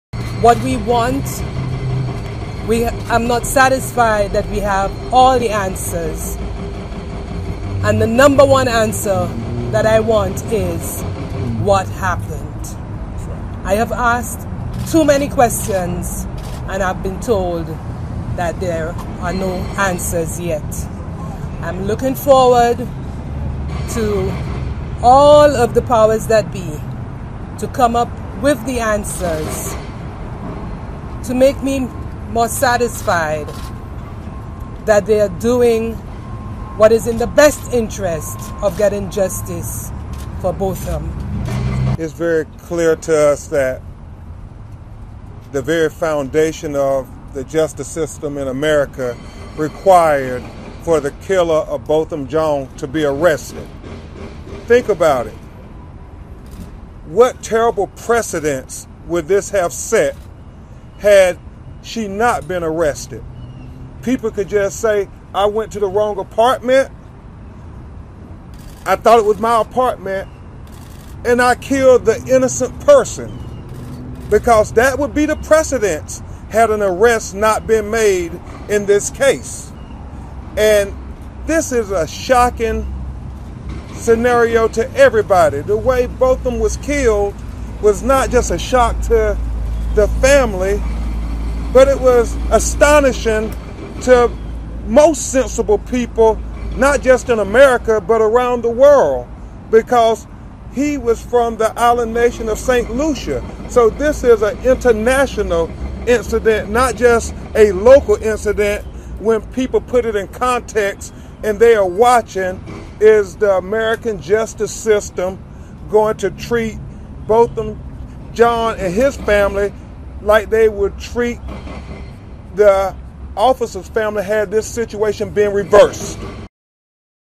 Mother Of Man Killed By Dallas Cop Seeks Answers
September 10, 2018 - The mother of a man who was gunned down inside of his Dallas home by a police officer who said she mistook his apartment for her own says investigators still haven't given her family an account of what happened. (Sept. 10)